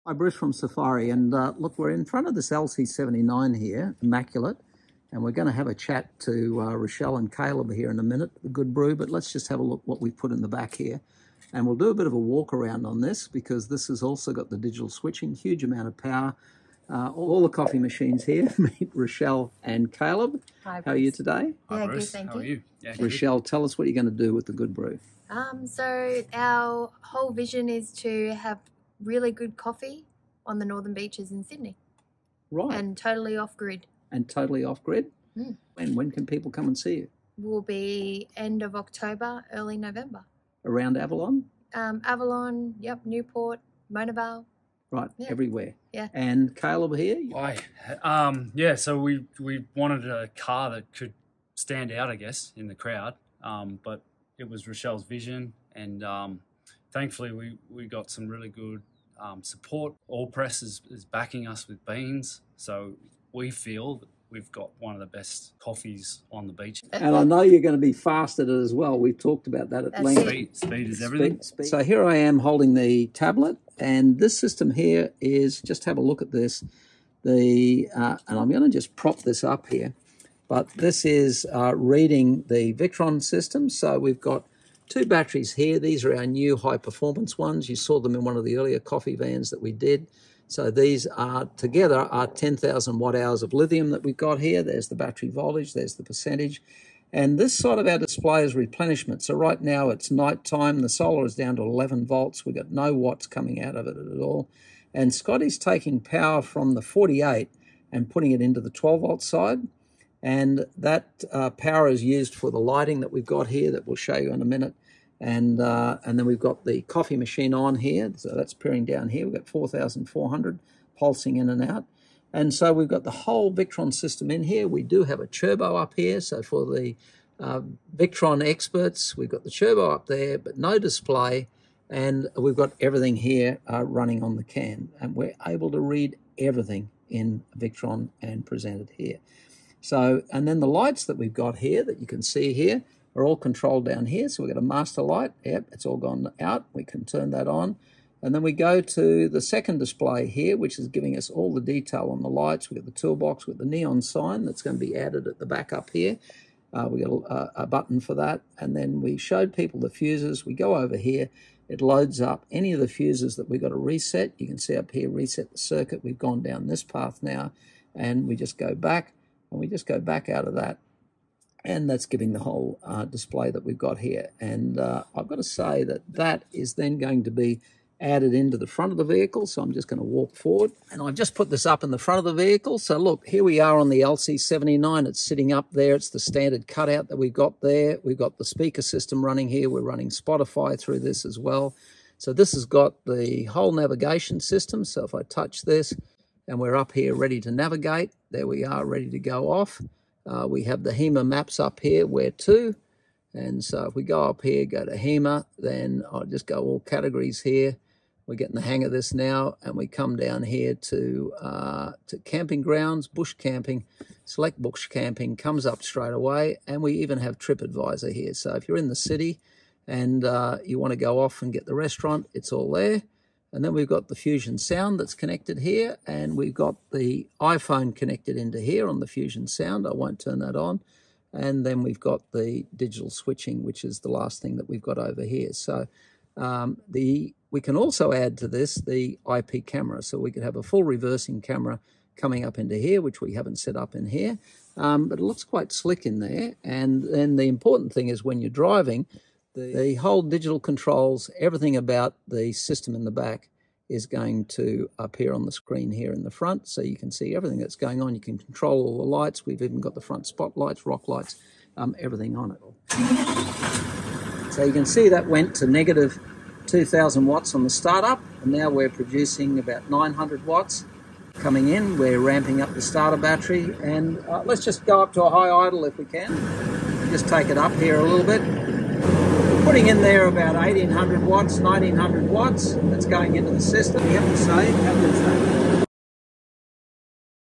LC79 Coffee truck with Safiery Integrated Display: Victron, Garmin head unit & Digital Switching
LC79 with Safiery Integrated Display: Garmin, Victron, & Digital Switching
Meet Rochelle and Caleb Moore of “The Good Brew” “coffee on the beech” for the Northern beaches of Sydney. Follow Rochelle @thegoodbrew.co and try the coffee plus see the final fit out. It will look completely different inside. I wont give away any of the exciting details. In this video we walk through the integrated system. The coffee machine pulls 4430W continuous, easily matched with the 6000VA Inverter/Charger and the 12000W of solar on top.  This is 800Ah Lithium (12V equivalent) charged up to 3,000W from Scotty and the vehicle’s upgraded 250A alternator. The Safiery Integrated Display sits on top of the Garmin Vieo Head Unit. This is Multi-function: Navigation, HEMA, Campgrounds, Fusion Sound, Victron Energy integration, Digital Switching, Tyre Pressure monitoring, and airbag control with the Inclinometer: Pitch and Roll. This system will automate airbag control to the correct flat level pitch!
The Safiery Integrated Display captures ALL the data from Victron using CANbus. This is NOT an NMEA output connection. This is a proprietary Safiery interface. All in the same Integrated Display BUT the functions are in separate devices so there is no single point of failure.
Safiery Integrated Removable Display Includes:
- Garmin Navigation with accurate GPS
- Garmin "Where to" Map Database
- Hema Maps
- Camping Grounds
- Dump Stations
- Reversing camera Input + 3 other Camera Inputs
10 inch High resolution Display (take to the fire & use)
Magnet second mounting pad for Display
Fusion Multi-media System (Bluetooth streaming & Airplay)
- 4 Channel Fusion Speaker Ready system (speakers extra)
Digital Switching of 20 outputs - no fuses needed 10A a channel
- Combine channels for greater than 10A - up to 40A
Software Licence (perpetual) for the following when Victron system purchased at the same time:
- Victron Connected Devices shown: needs a Cerbo GX (extra)
- Smart Solar, Multiplus inverter chargers, Smart Shunt (all extra)
- Displays CANbus reading Scotty with Alternator temperature
- Displays CANbus reading 12V or 24V Lithium Batteries directly
- Tyre Pressure Monitoring up to 12 Tyres of pressure and temperature (TPMS is extra)
- Inclinometer and Airbag control (Items extra)
- Solar irradiation sensor ready (shows when panels need cleaning)
- IP Camera Ready (up to 8)
- Up to 8 tanks ready through Victron System
- Up to 8 temperatures (combo may need CAN based Temp Sensors)